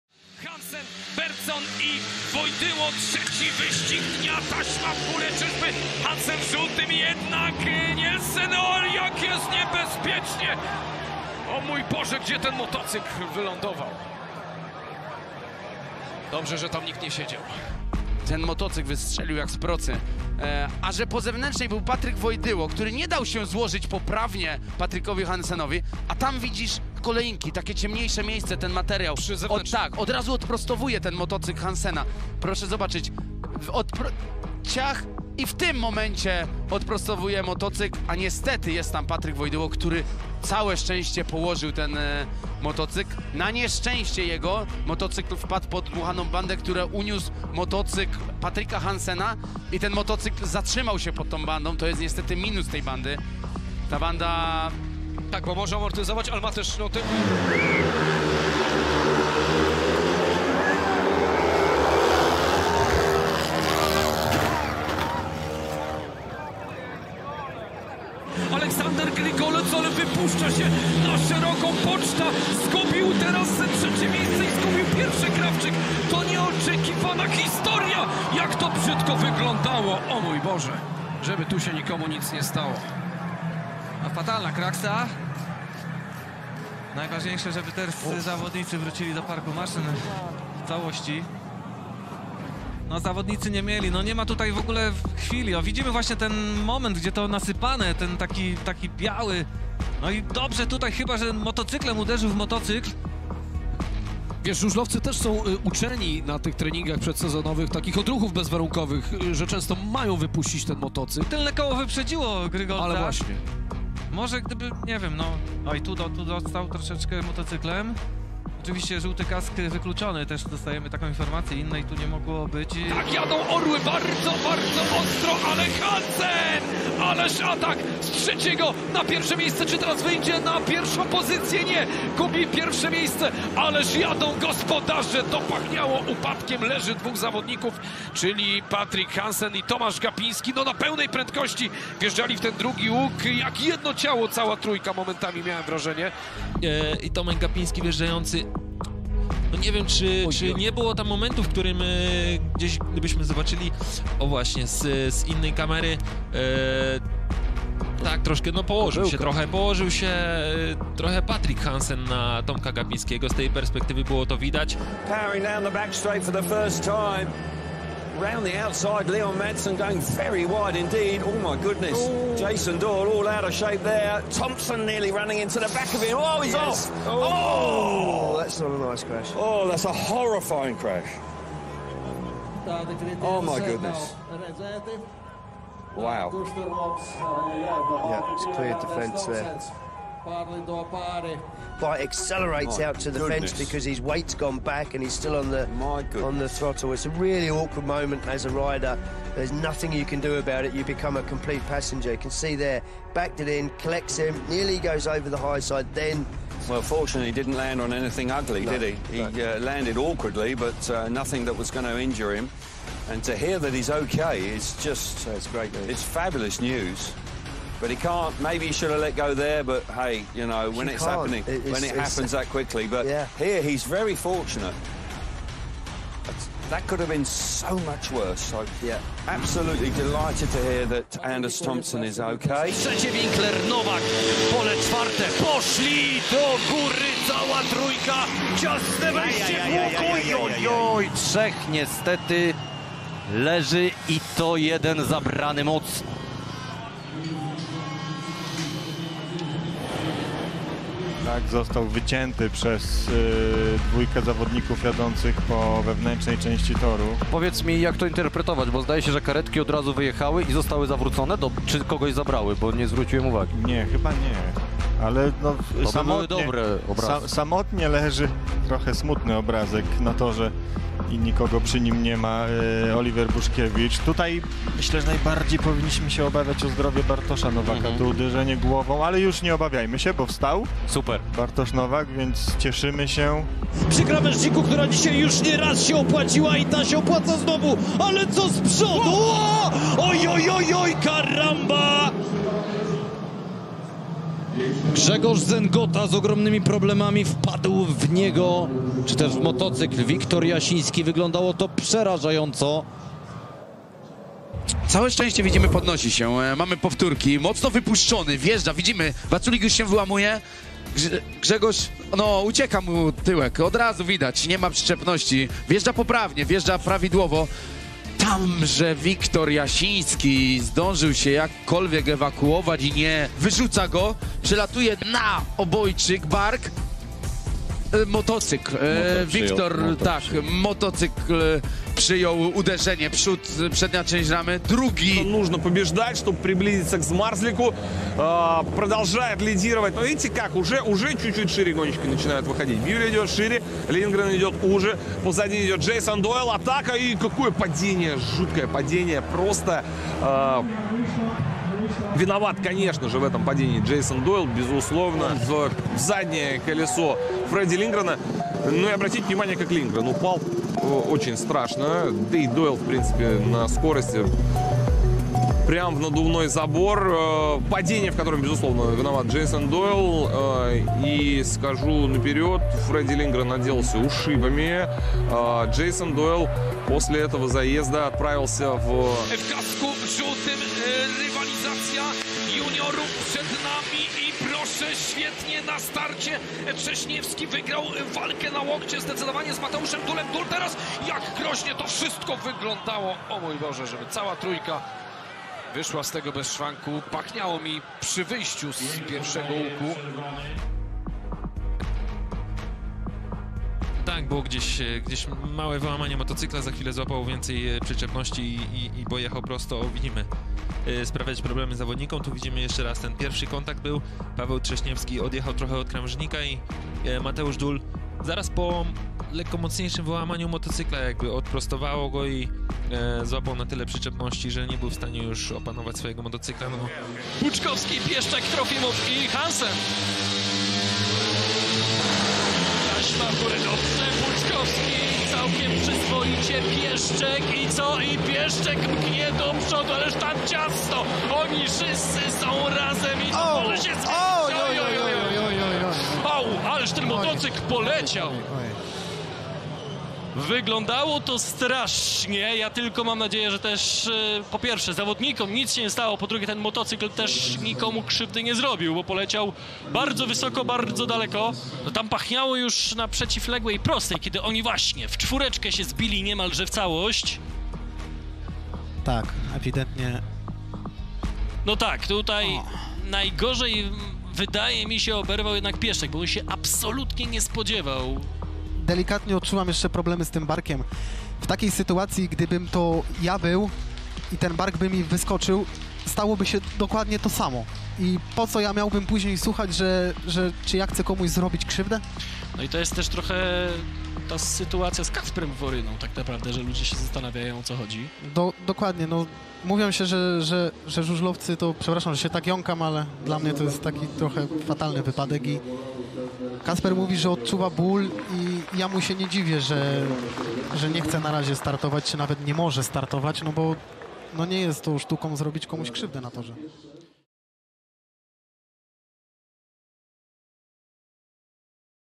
Worst Speedway Crashes of 2023 (Speedway Upadek)
0:00 1 Liga. Ostrow
1:08 1 Liga. Ostrow
2:03 1 Liga. Lodz
2:44 Speedway GP. Riga
3:52 1 Liga. Rybnik
4:44 PGE Ekstraliga. Gorzow
5:35 Speedway GP. Vojens
6:19 1 Liga. Lodz
7:07 1 Liga. Rybnik